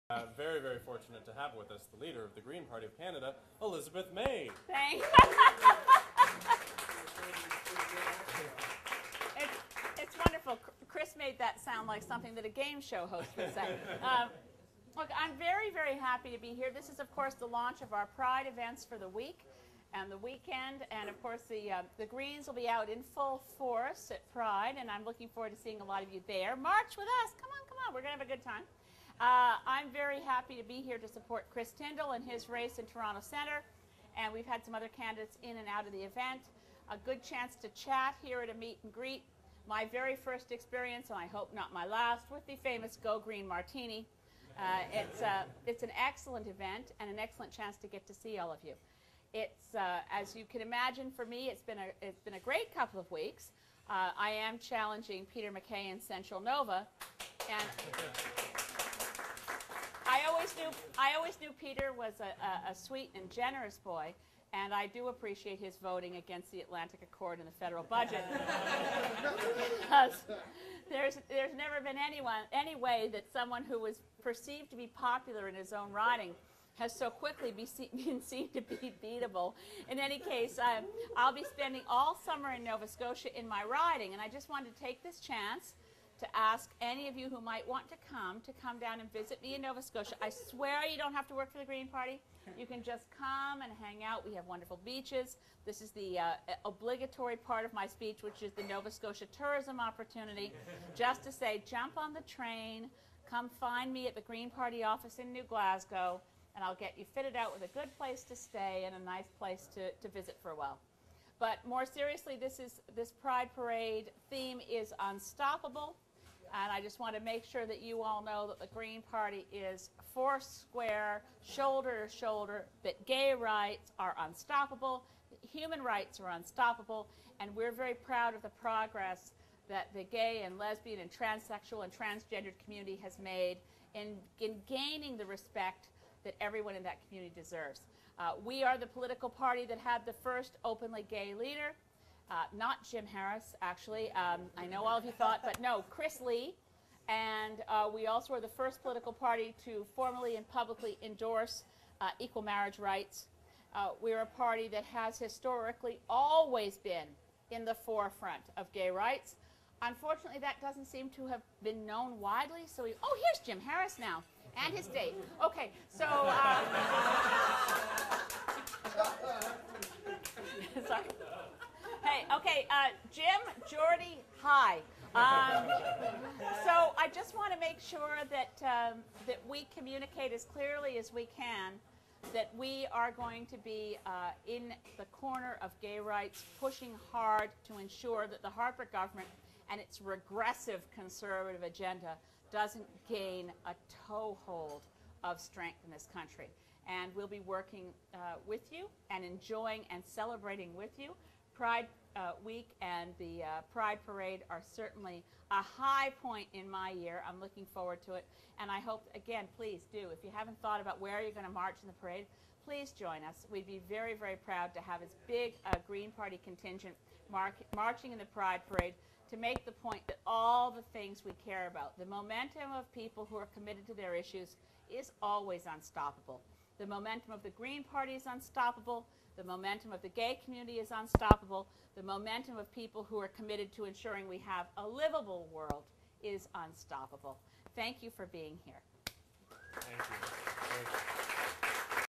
Elizabeth May at Pride Week 2007
Elizabeth May, leader of the Green Party of Canada, re-affirms her party's commitment to gay, lesbian, bisexual, transsexual and transgendered rights on Church Street with Toronto Centre candidate Chris Tindal.